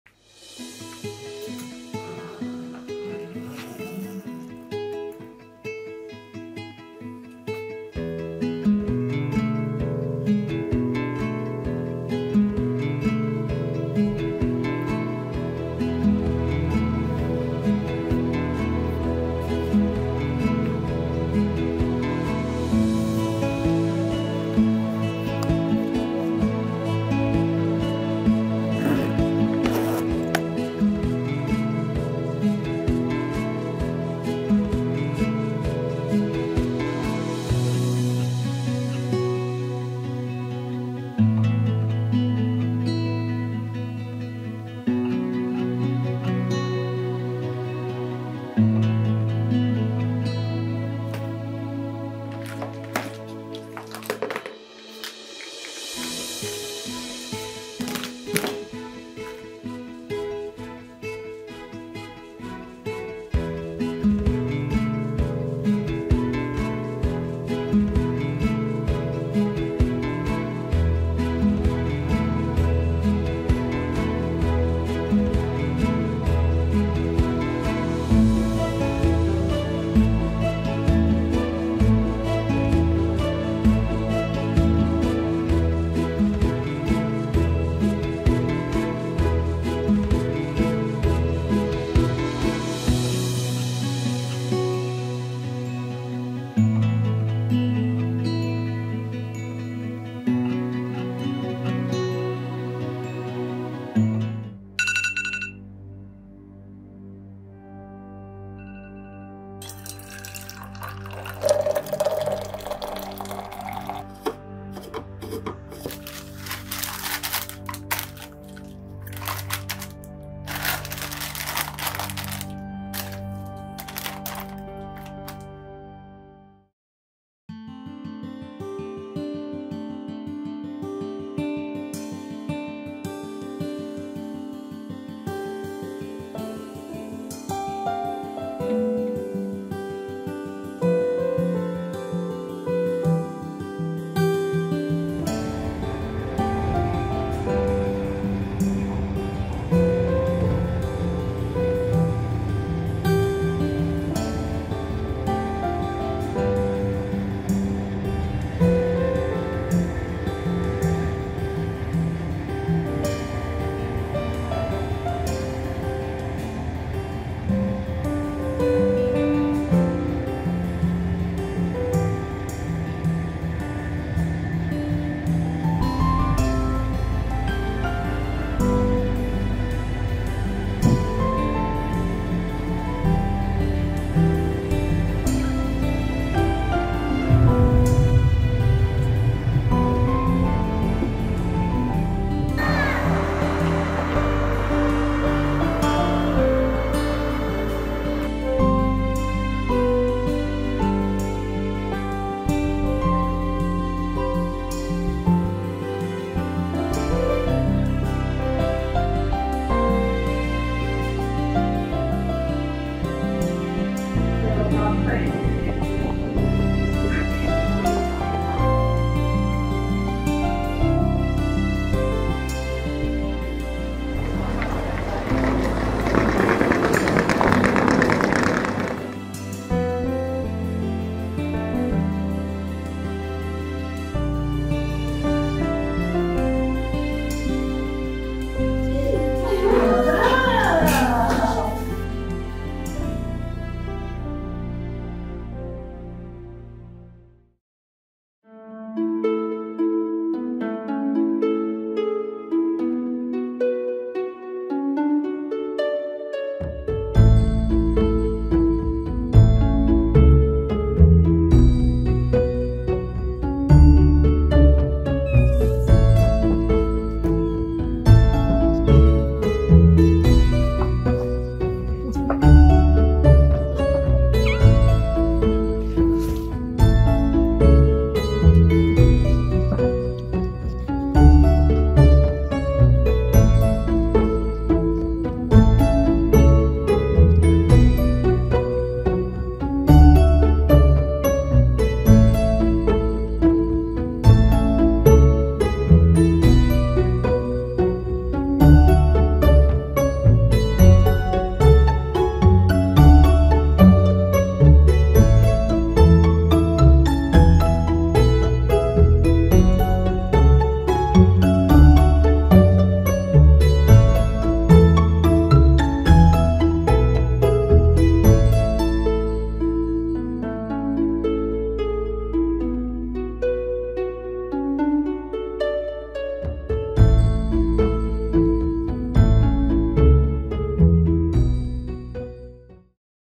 KIRIKKALE YOLCUSU KALMASINNN / HIZLI TREN / (SESSİZ VLOG)
Merhaba arkadaşlar bugün ki videomuzda yeni bir sessiz vlog ile karşınızdayız. Kırıkkale'ye yolculuğumuzu kısa bir süre içinde ve sizleri sıkmadan paylaşmak istedik. Umarım beğenirsiniz. İyi seyirler diliyorum.